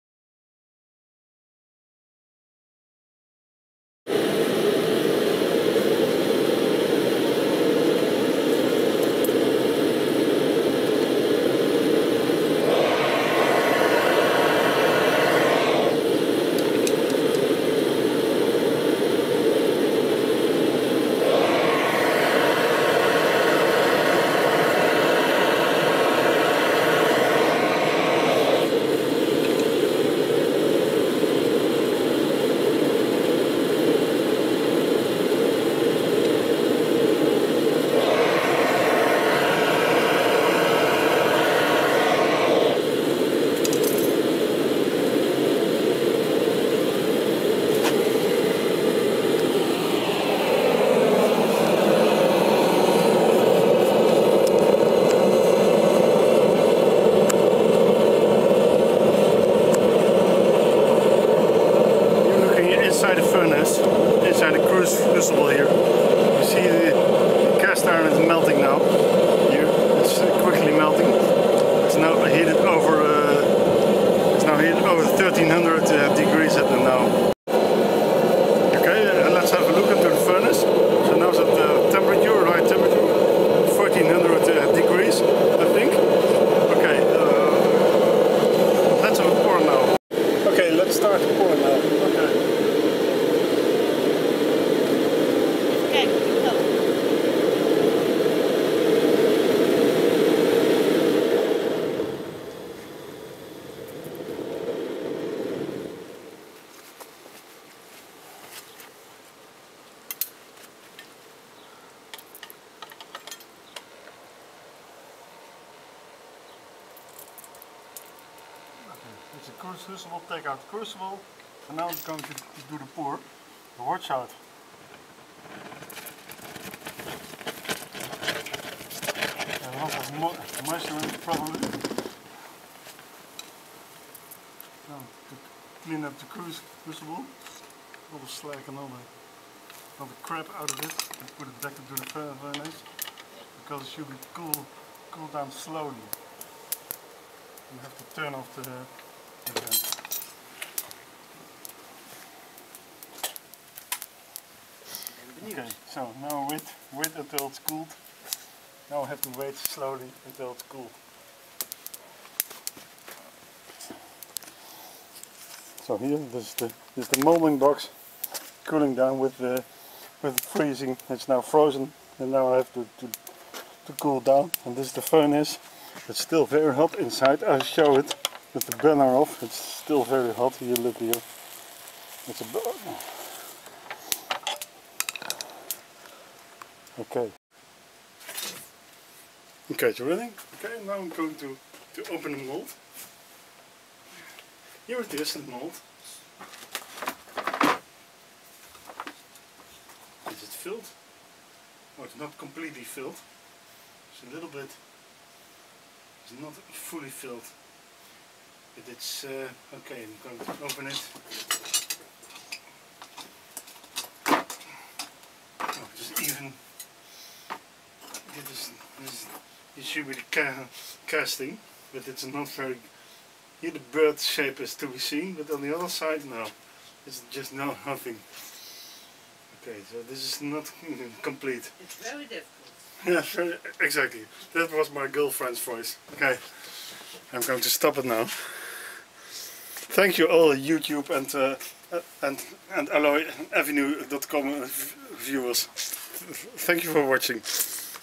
Another try of iron casting
Melting and castting a small amount (399g) of cast iron.
Unfortunately, the mold did not fill completely.